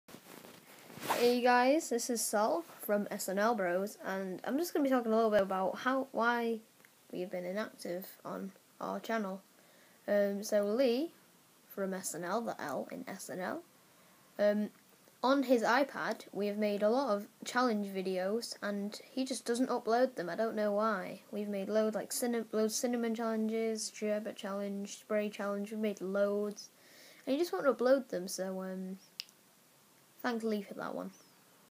Why SnL Have Been Inactive (READ DESCRIPTION)
Sorry for being inactive but from now on, Lee is going to be uploading a challenge once a day for about a week, thanks guys
- Sol